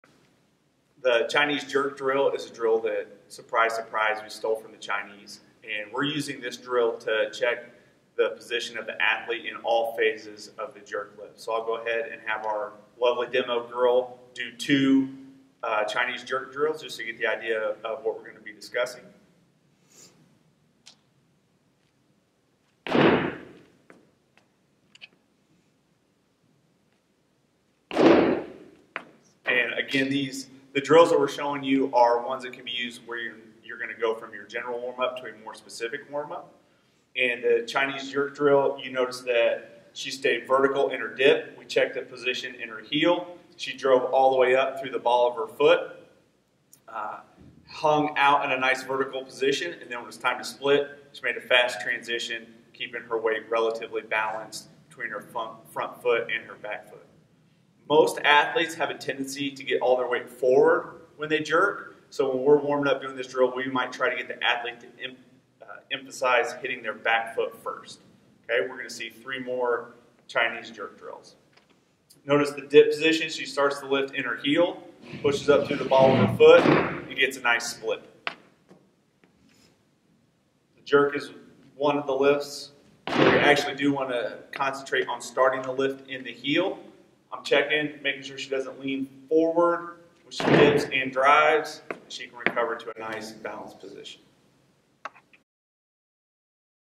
Chinese Jerk Drill
Do you know where the Chinese Jerk Drill Came from? Dan will tell you.